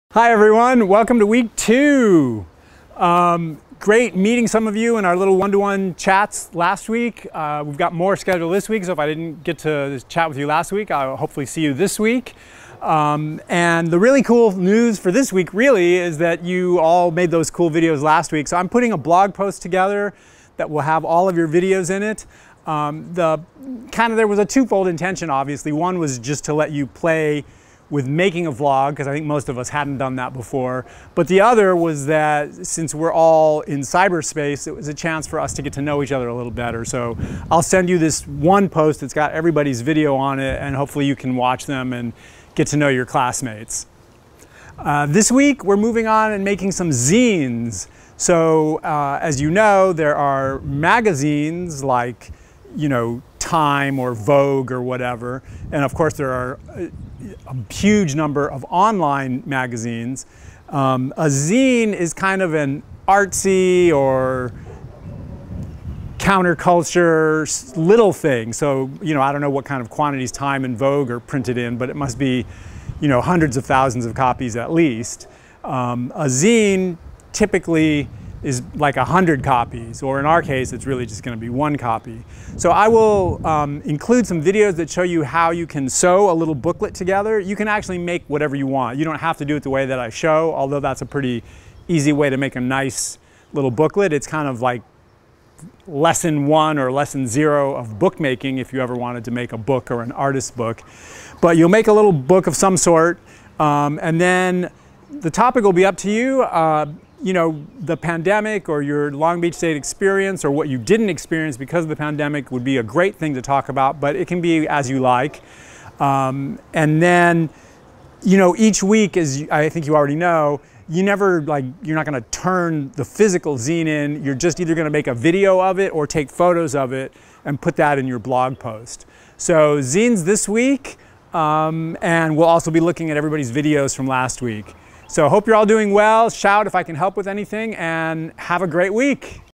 Making Zines! Week 2, Summer 2021, School of Art, College of the Arts, Long Beach State University!
This week we'll make some cool zines.
Check out some great artists.
And take a look at everyone's vlogs from last week.
Have a great week and shout if I can help with anything!